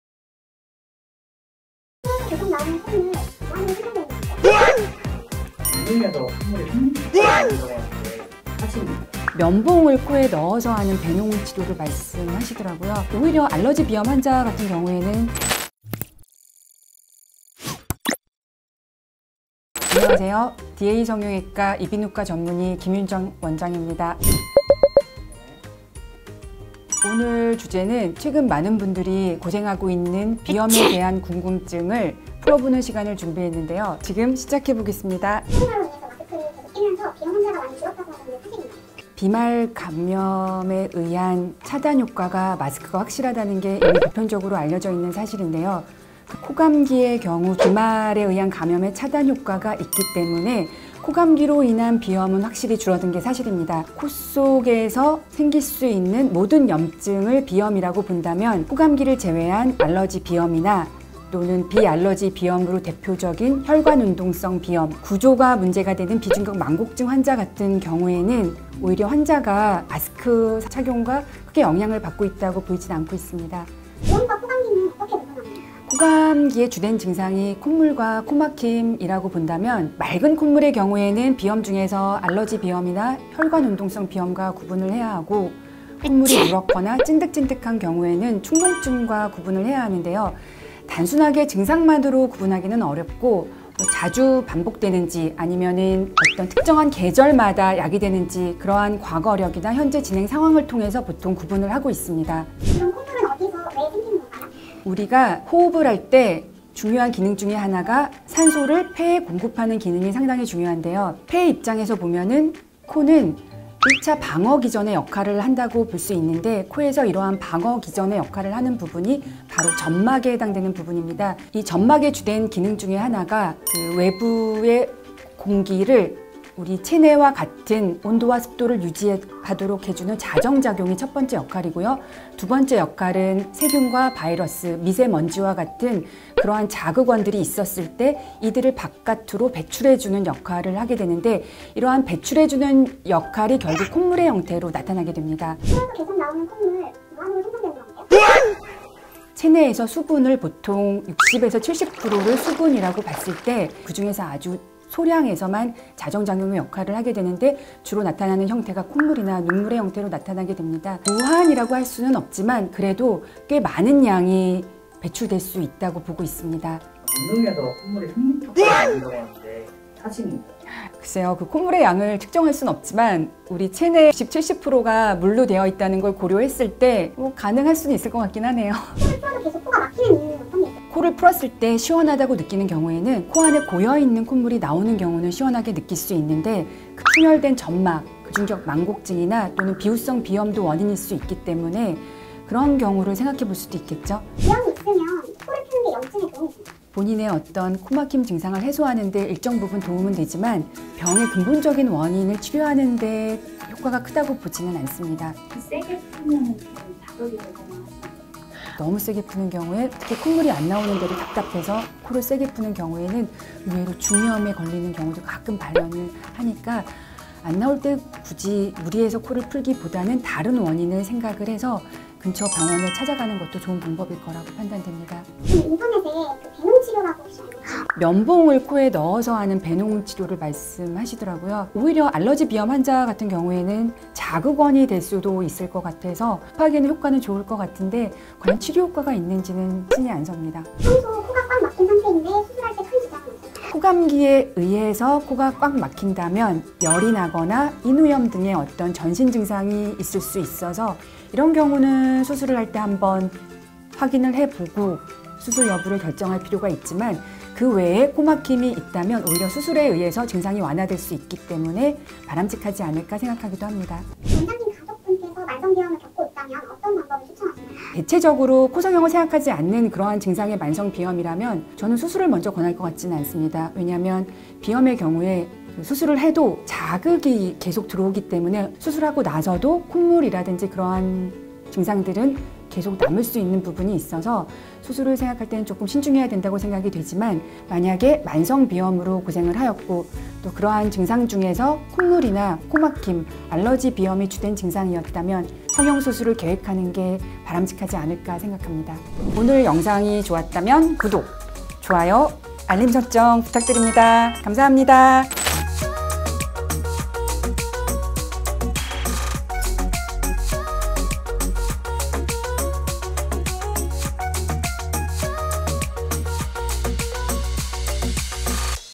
[비염 낫는 법] 콧물이 3L까지? 코감기, 비염, 축농증 구분법! 코수술 비용 [디에이성형외과]
콧물이 최대 3L까지
나온다는 소문이 사실인가요?
비염과 마스크의 상관관계는요?

이비인후과 전문의가 알려주는
비염 없애는 법 팩트 체크와
코감기, 비염, 축농증 구분법까지!

0:00 intro
0:20 인사
0:35 마스크와 비염의 상관관계
1:14 비염과 코감기 구분법
1:42 콧물은 왜 생기는 건가요?
2:47 콧물이 3리터?
3:14 코를 풀면 염증에 도움이 되나요
3:45 배농치료 효과 있나요
4:01 코가 꽉 막힌 상태인데 코수술시 지장 없나요.
4:24 원장님 가족들이 만성 비염을 겪고 있다면